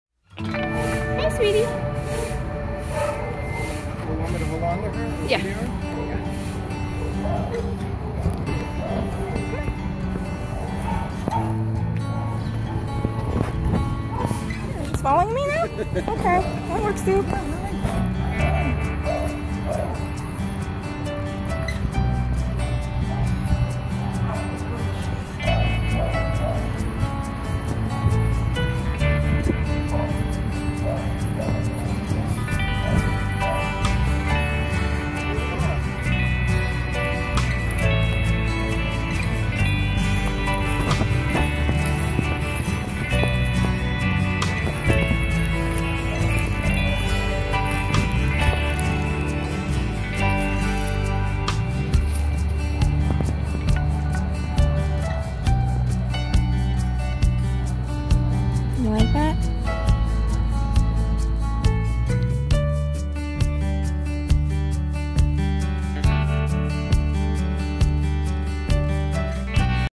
A1622275 TINY *RED ALERT URGENT!!*
Tiny (A1622275) is a 3 year old, spayed female, fawn and white American Pit Bull Terrier and Bull Terrier mix. She weighs approximately 45 pounds and has been at the shelter since April 20, 2016.